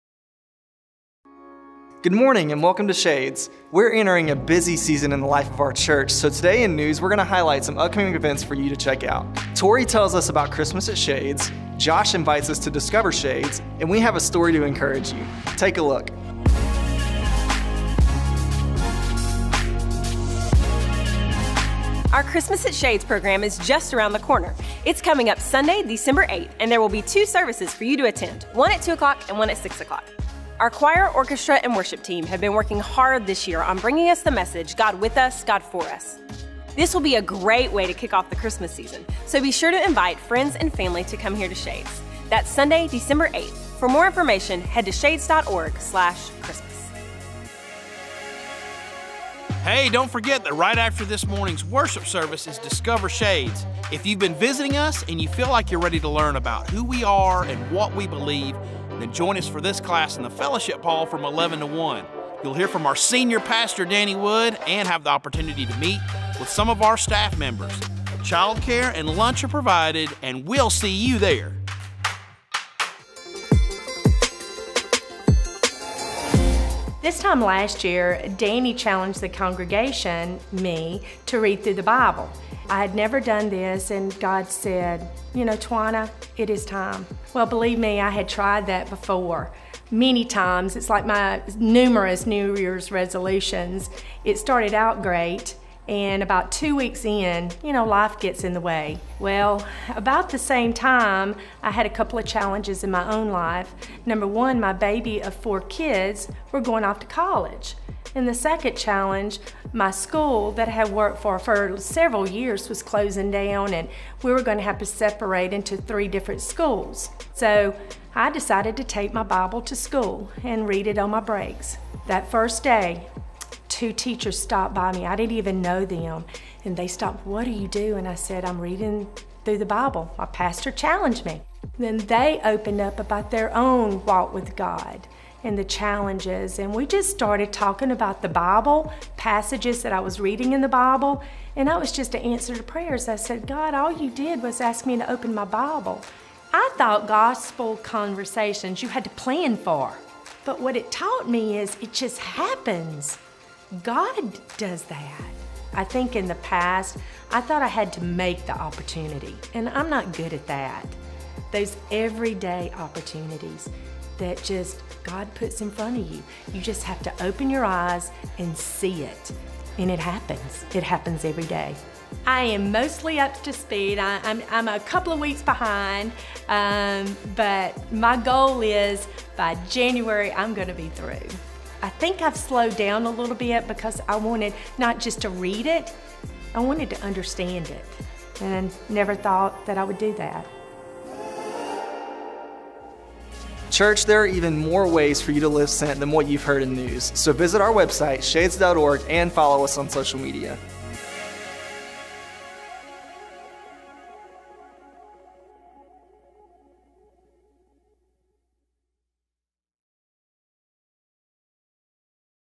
Shades News | Ep. 40
It's hard to believe we're in the home stretch of 2019. With Thanksgiving and Christmas right around the corner, here's what's happening at Shades.